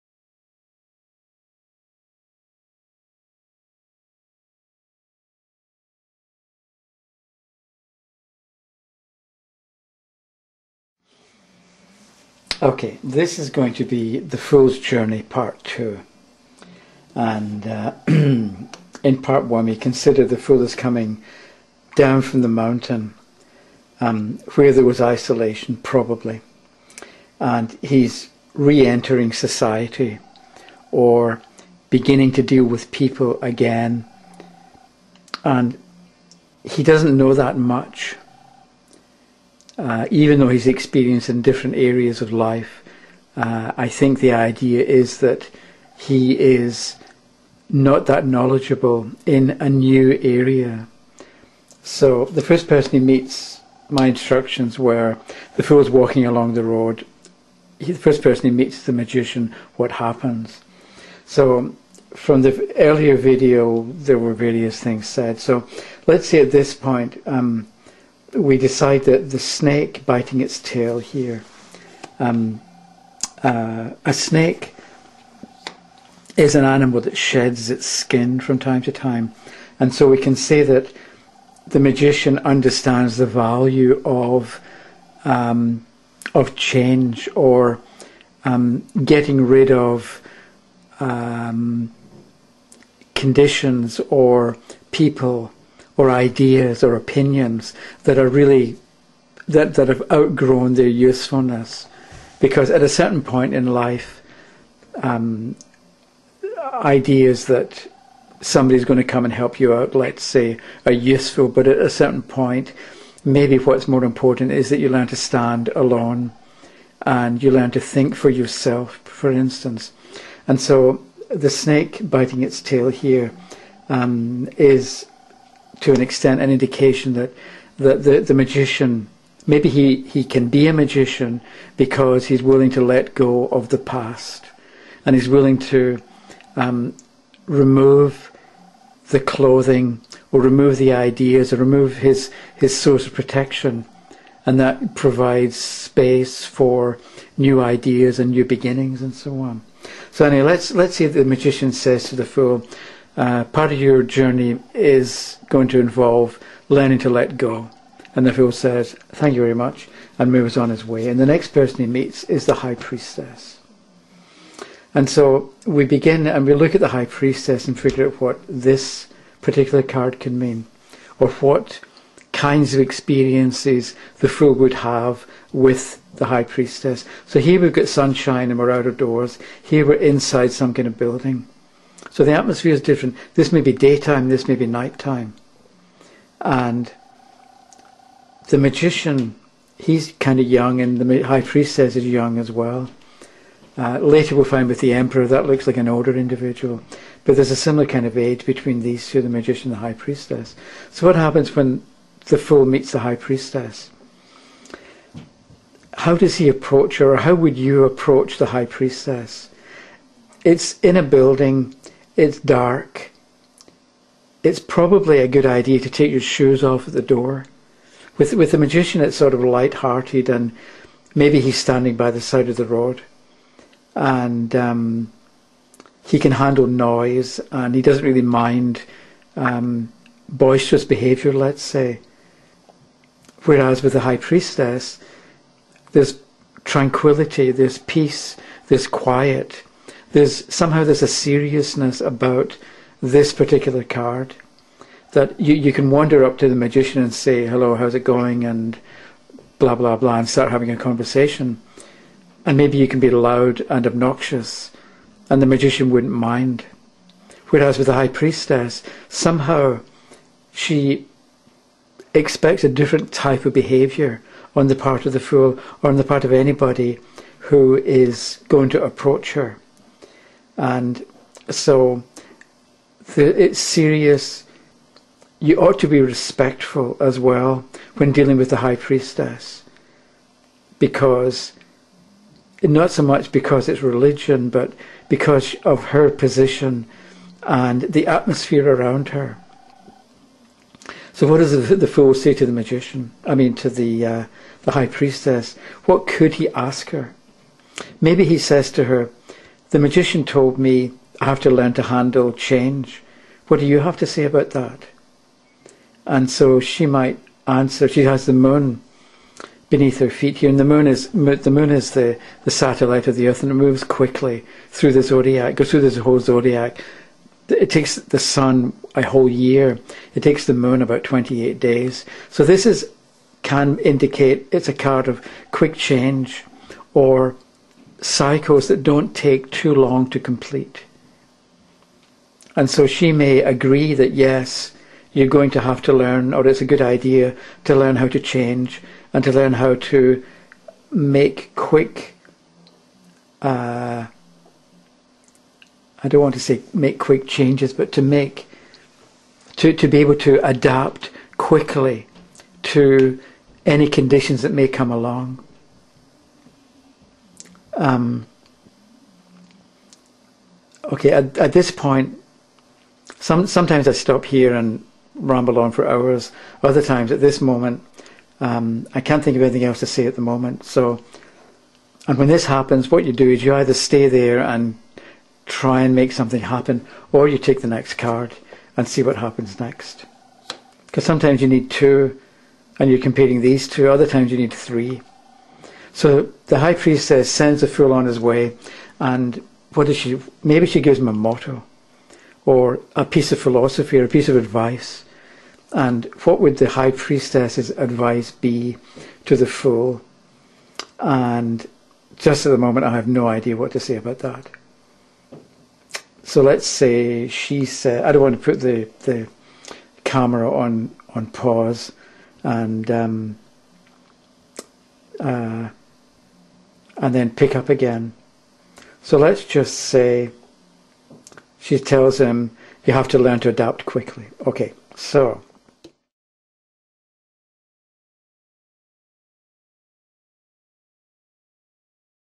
fThe Fool's Journey 2: Part One of Two
We follow the Fool as he meets next the High Priestess, the Empress and the Emperor.